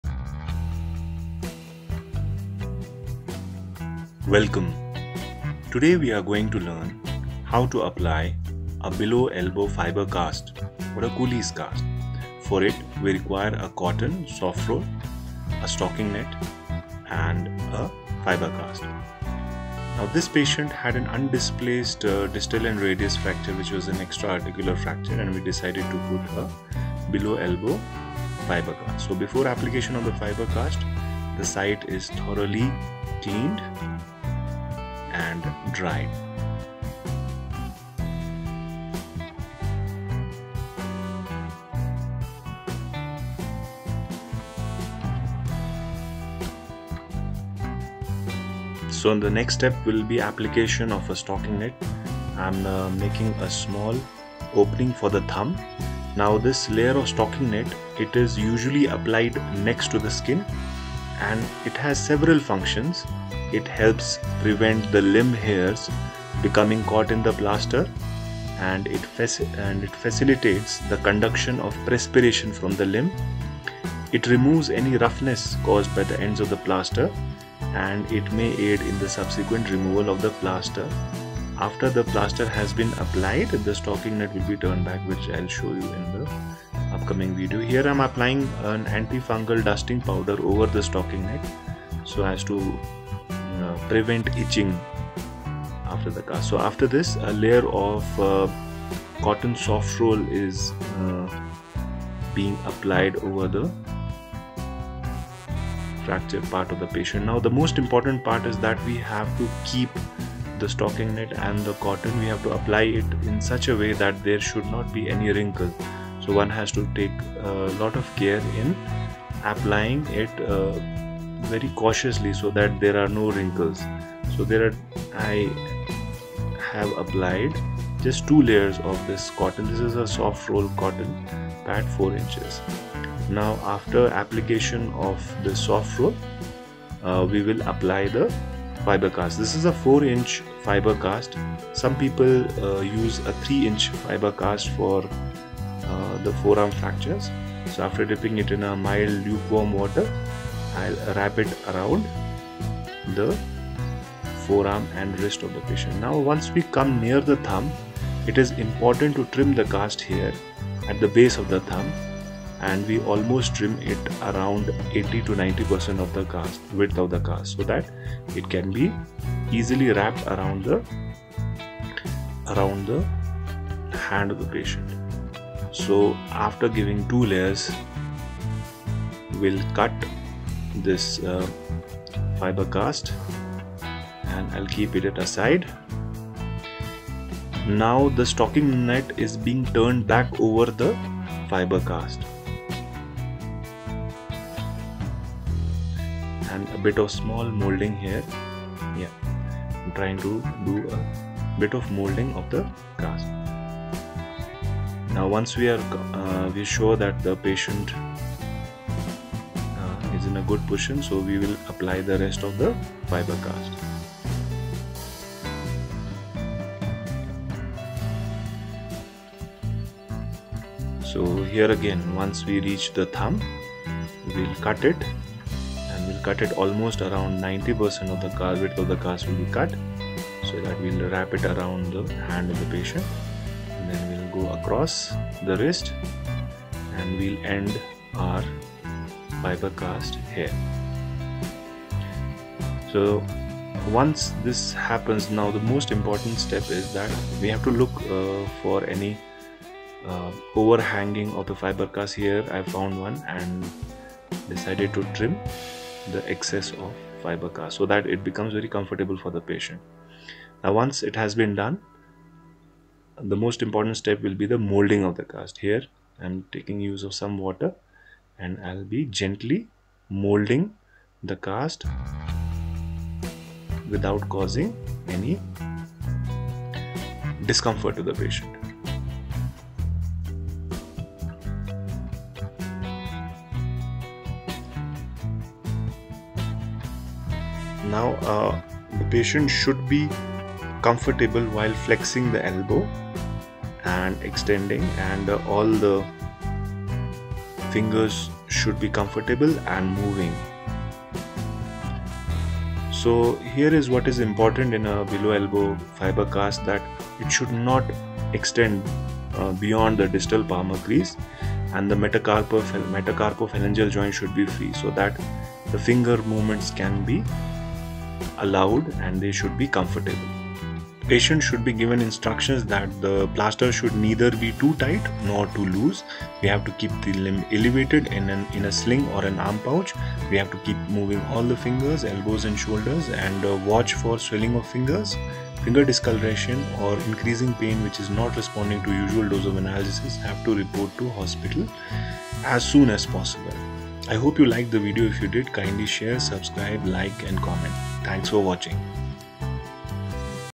LEARN HOW TO APPLY A COLLES CAST FOR DISTAL RADIUS FRACTURE
COLLES CAST FOR EXTRAARTICULAR DISTAL RADIUS FRACTURE